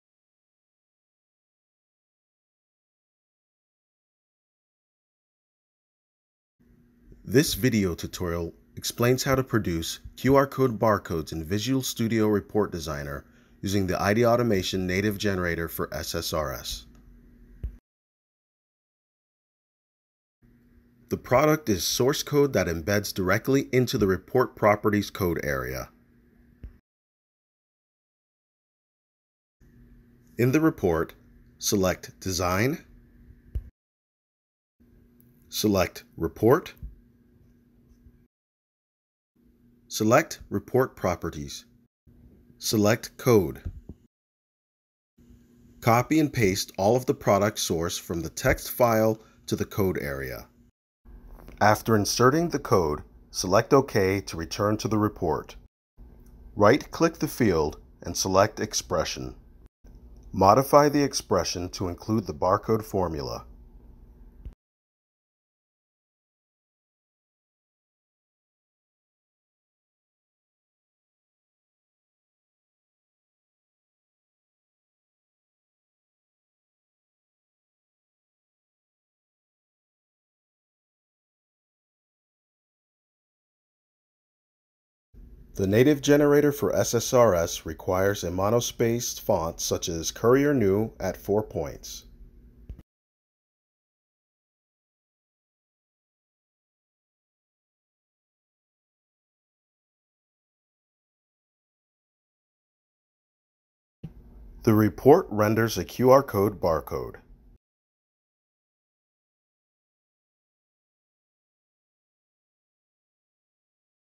How to create QRCode Barcodes using the Native Barcode Generator for SSRS
This tutorial explains how to produce QR-Code barcodes using the IDAutomation Native Barcode Generator for SSRS

Formula name: =code.IDAutomation_SSRS_QRCode()

Compatible with ReportDesigner for VisualStudio and ReportBuilder. This product is extremely easy to implement and does not require you to reboot your servers to display the barcode. It doesn't use any DLLs for barcode fonts, only code, and the Courier New Font at 4 points for 2D barcodes.